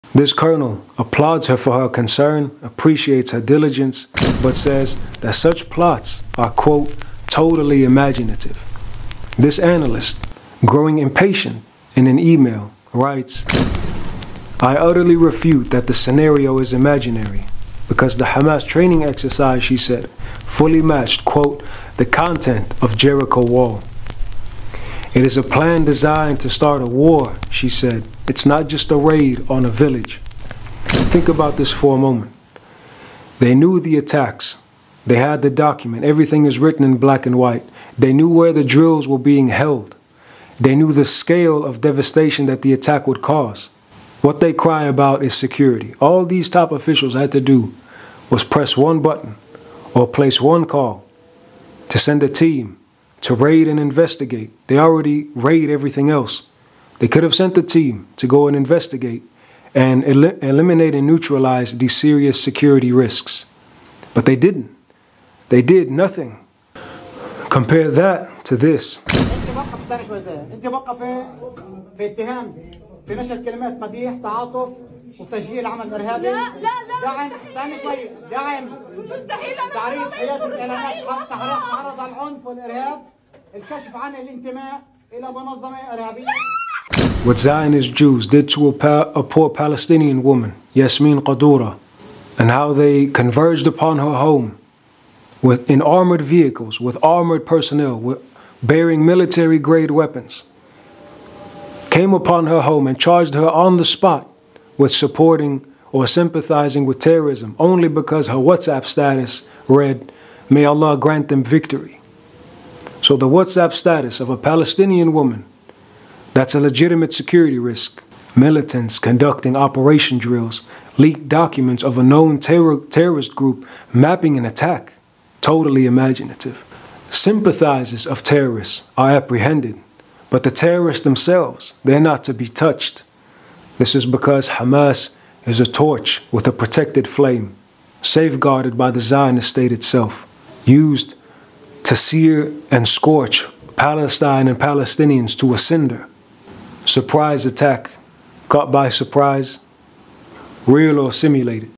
PROOF the TRUE target was ALWAYS Palestinians, Not H*MAS
Z!0n!st state allows, evens feeds H*MAS to grow strong - let's them go free while they arrest and torture civilians for "sympathizing" with H*MAS. Brother Abdulwakil As-Siyari.